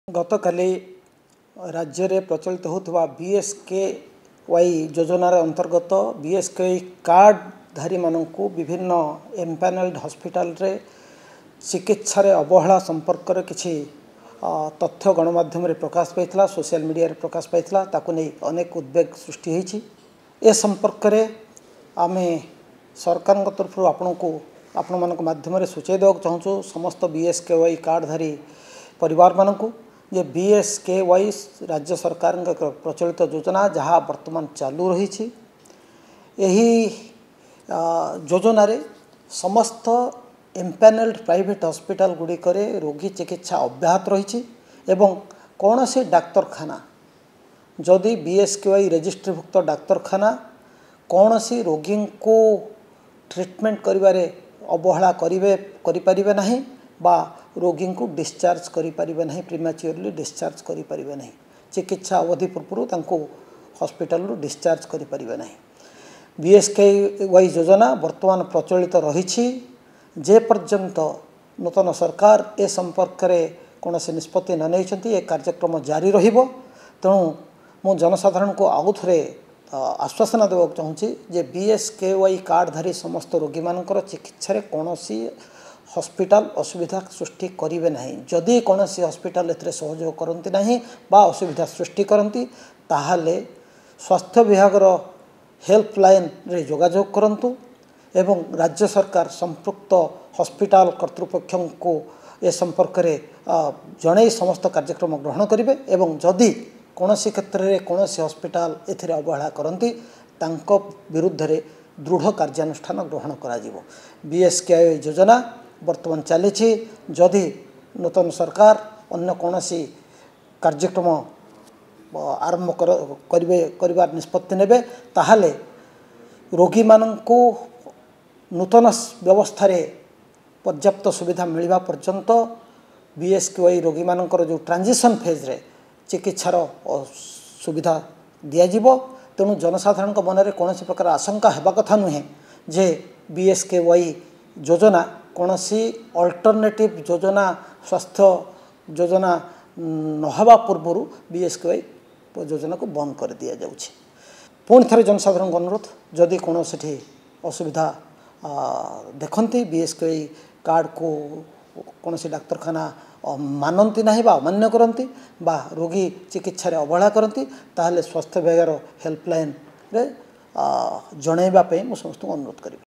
Cashless Treatment Facilities Under BSKY Remain Active: Chief Secretary Pradeep Jena
Stern action will be taken against hospitals denying treatment facilities to BSKY beneficiaries.

All BSKY beneficiaries will continue to avail cashless healthcare services. All empanelled hospitals have been asked to continue treatment to beneficiaries as before. People can reach out to the health dept via its helpline number about any hospital denying or neglecting treatment facilities: Odisha Chief Secretary Pradeep Jena.